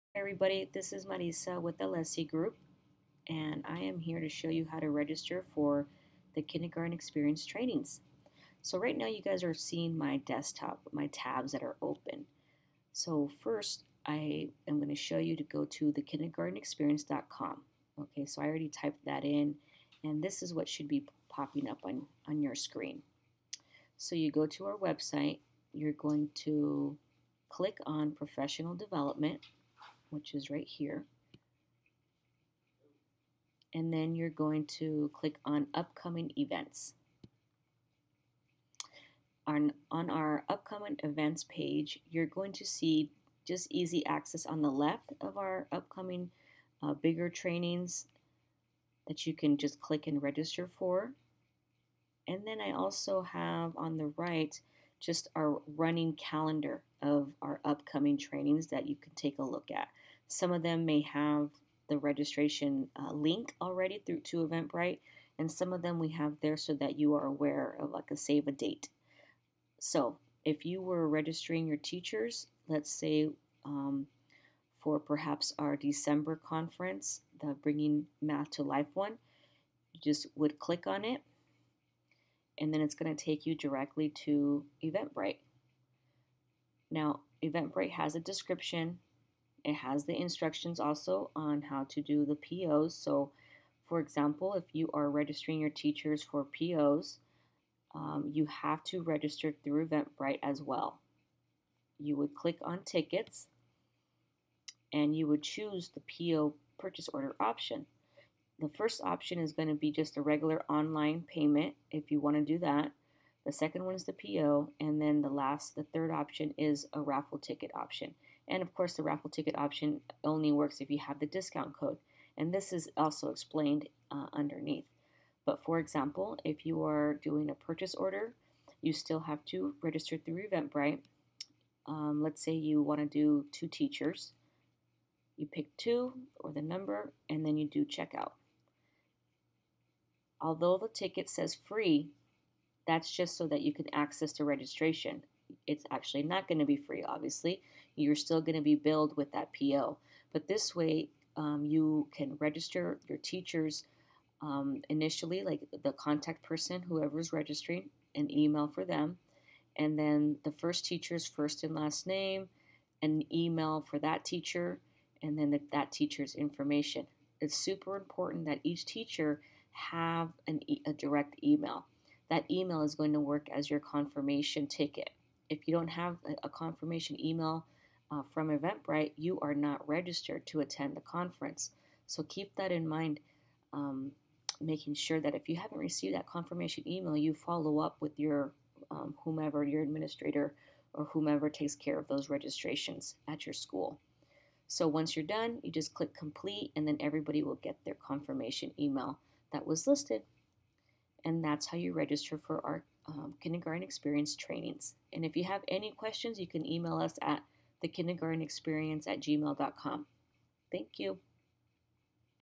How To Register for a Kindergarten Experience Training
How to register for an Alesi Group training for The Kindergarten Experience.